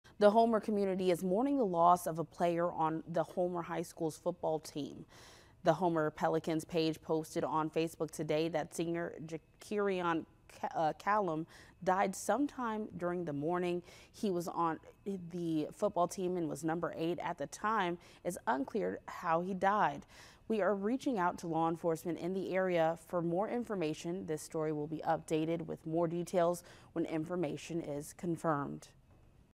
Homer High School football player dies
Ja’Kerion Calome died sometime during the morning. He was #8 on the team.